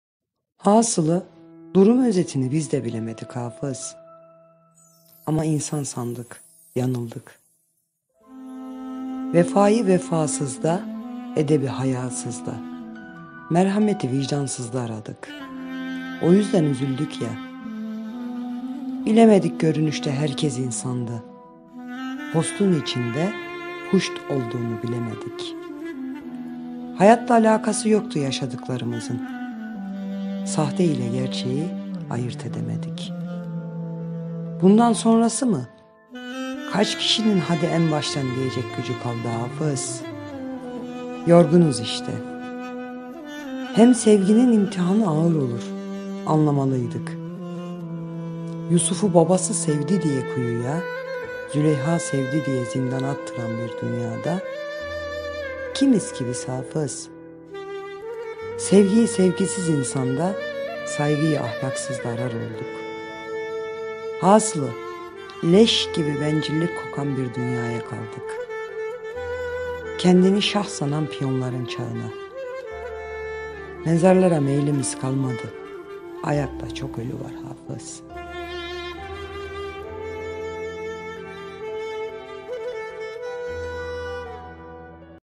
Şiir | insansanız
Hasılı durum özetini bizde bilemedik hafız
Ama insan sandık yanıldık
Vefayı vefasızda
Edebi hayasızda
Merhameti vicdansızda aradık
Oyüzden üzüldükya
Bilemedik görünüşte herkes insandı
Postun içinde puşt olduğunu bilmedik
Hayatla alakası yoktu yaşadıklarımızın
Sahte ile gerçeği ayırt edemedik
Bundan sonrasımı kaç kişinin hadi en baştan diyecek gücü kaldı hafız
Yorgunuz işte
Hem sevginin imtihanı ağır olur anlamalıydık
Yusuf'u babası sevdi die kuyuya
Züleyha sevdi diye zindana attıran bir dünyada
Kimizki biz hafız
Sevgiyi sevgisiz insanda
Saygıyı ahlaksızda arar olduk
Hasılı leş gibi bencillik kokan bir dünyaya kaldık
Kendini şah sanan piyonların çağına
Mezarlara meylimiz kalmadı
Ayakta çok ölü var hafız...Meltem Klkn